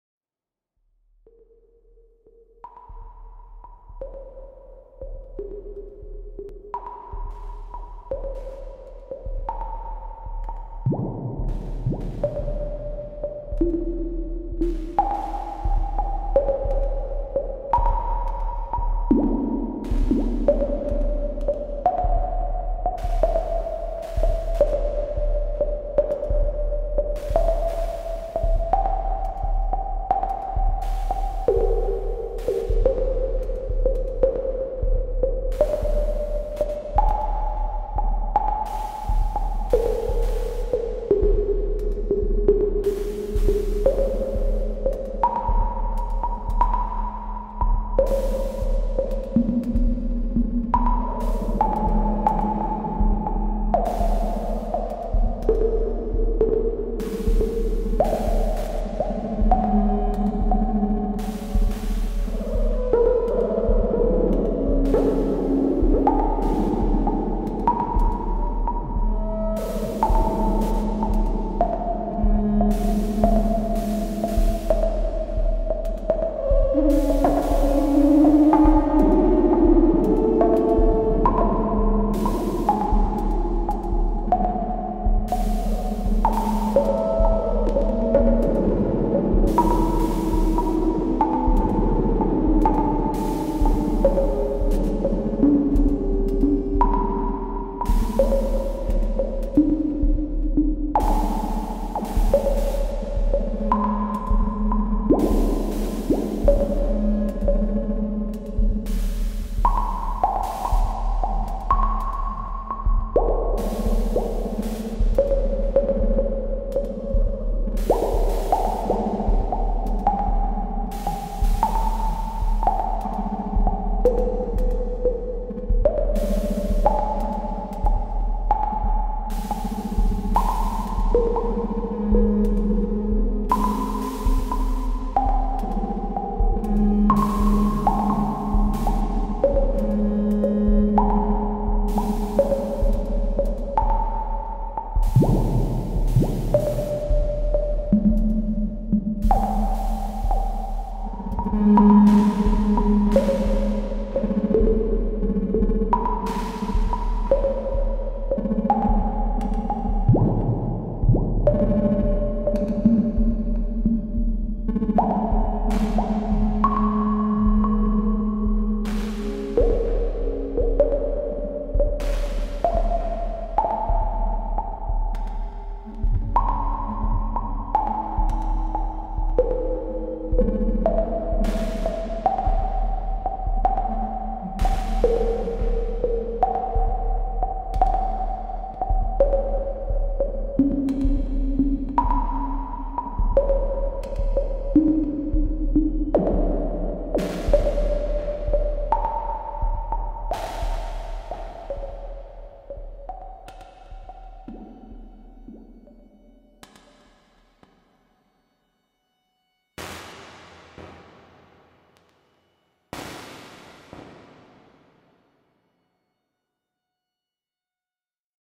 Batchas presents: Plumbutter - Patch 003
Impro on the Plumbutter 1.
Direct outs, one take.
Added some reverb afterwards.